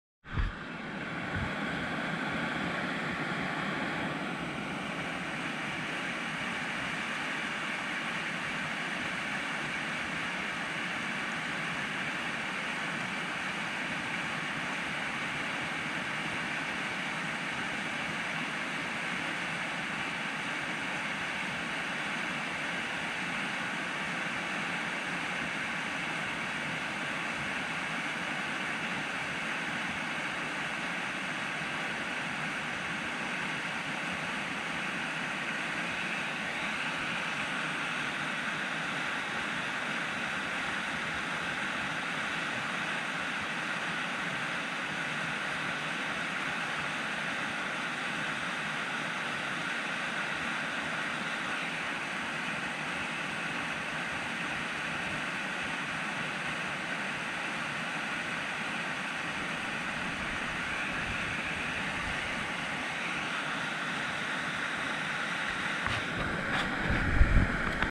Chetco River (Before the Gorge)
A lovely spot to enjoy the Chetco River, up before entering the Gorge. After a few seconds, you'll see a Swallowtail Butterfly fly past. They were everywhere!